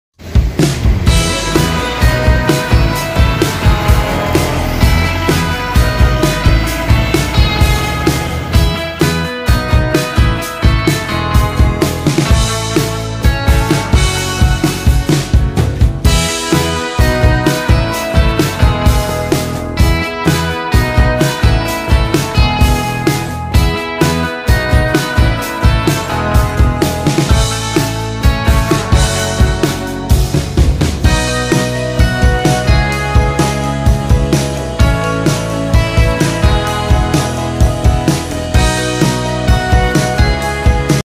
Penampakan BIS PATAS RESTU | Surabaya-Ponorogo
Bis patas Restu Surabaya-Ponorogo biayanya Rp 100rb